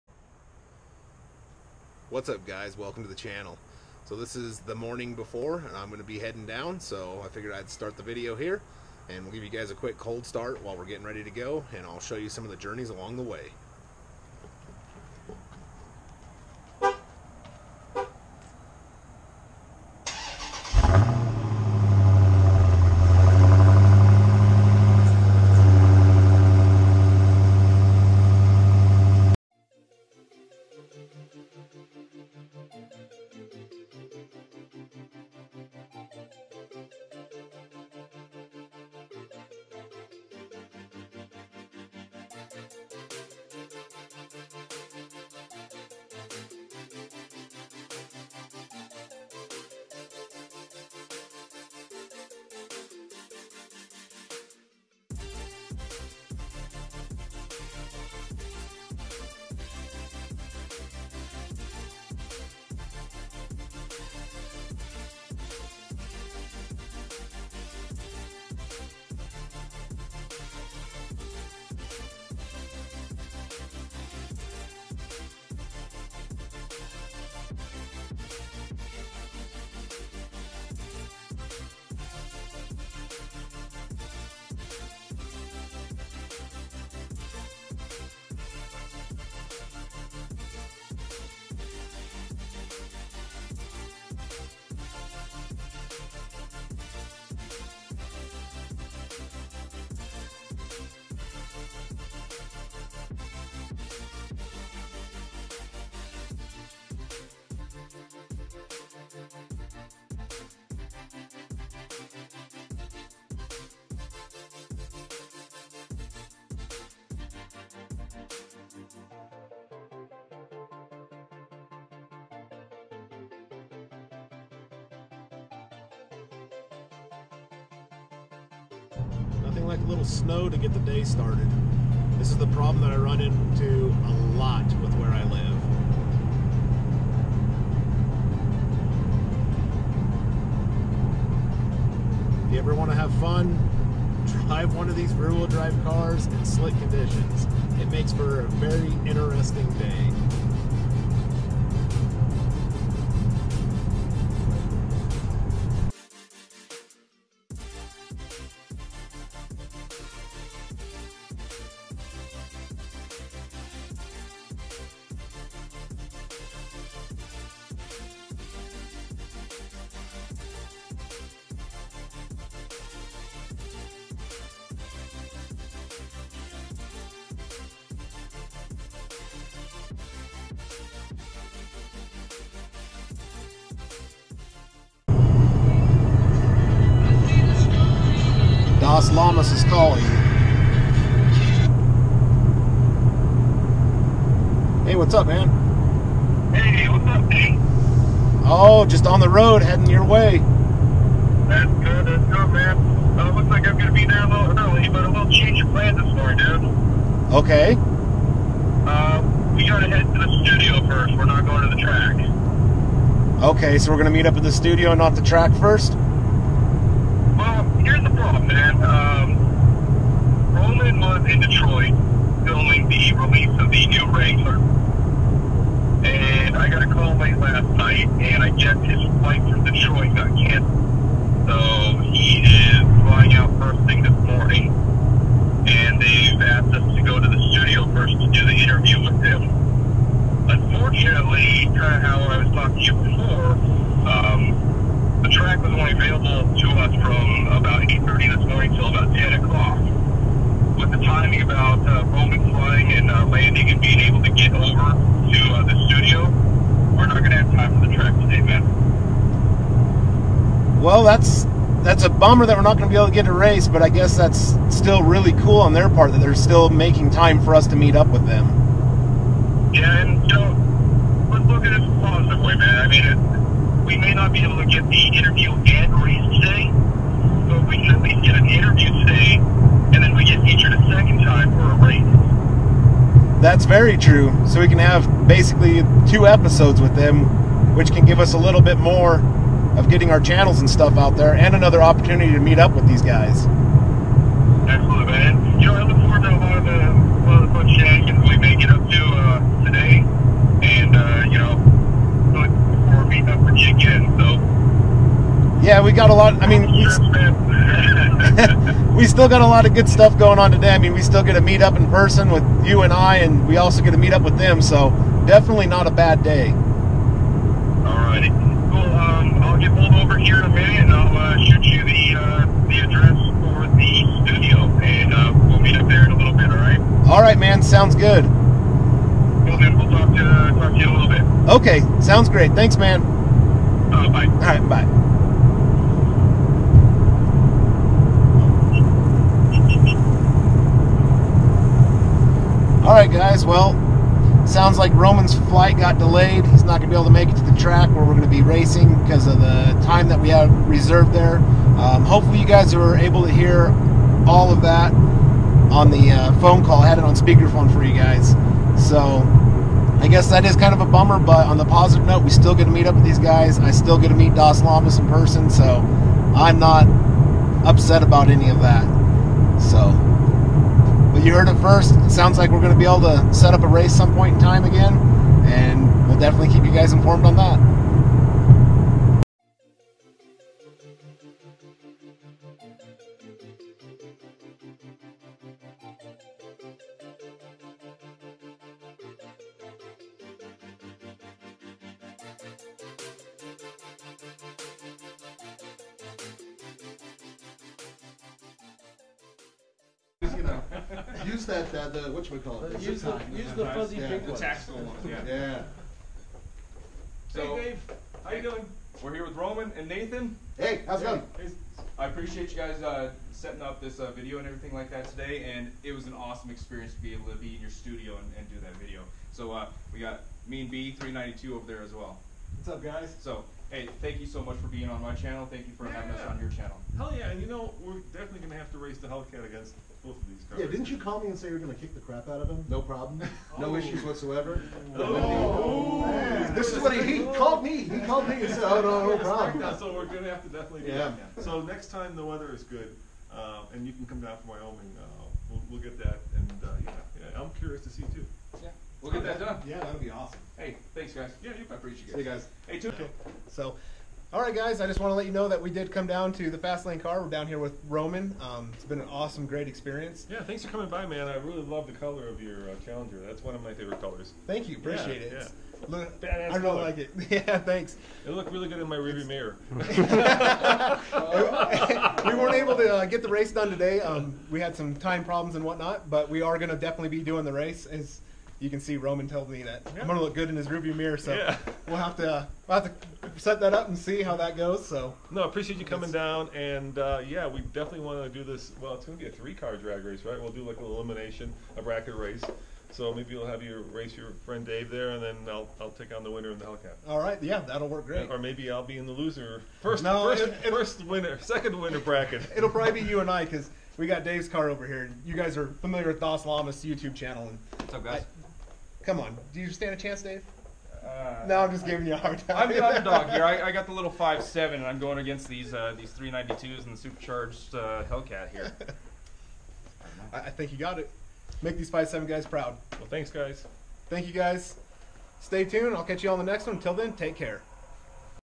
TFL car interview | Update on Hellcat race
Taking you guys along with me for the hellcat race but with plans changing along the way it still worked out for the best! Had a GREAT time with Dasllamas and The Fast Lane crew! At the end of the video I talk with Roman and you guys don't want to miss that!